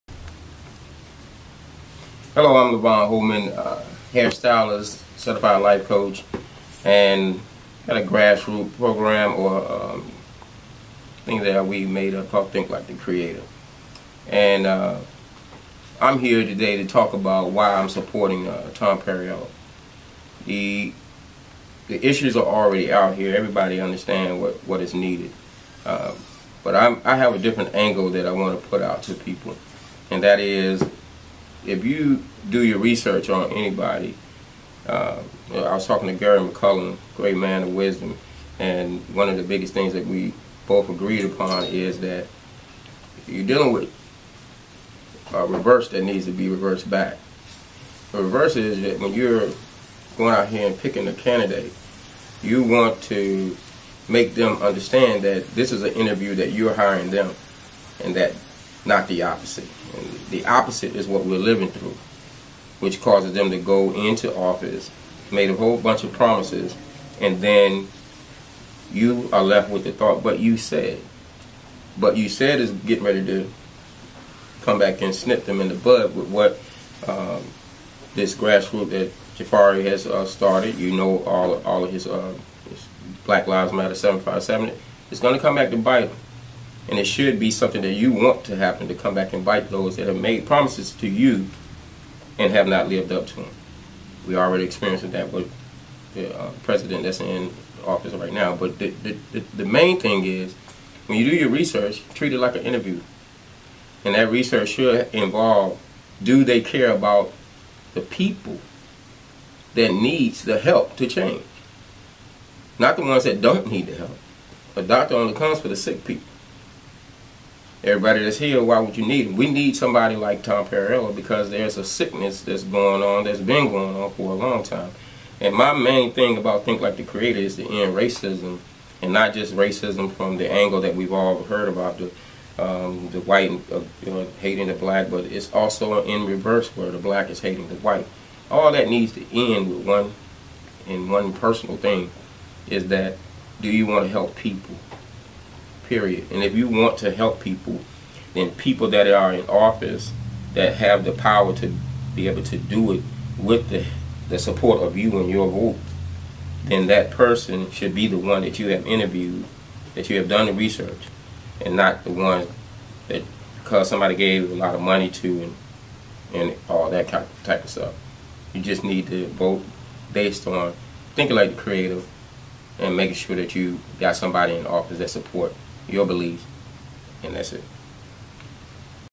Think Like The Creator supports Tom Perriello!
Primary1613 "Think Like The Creator" founder & award winning hair artist Levon Holman challenges everyone to take a someone spiritual approach when HIRING an elected official! He also gives his reasonings for his support of Tom Perriello for Governor! (Please share) BlackLivesMatter757.org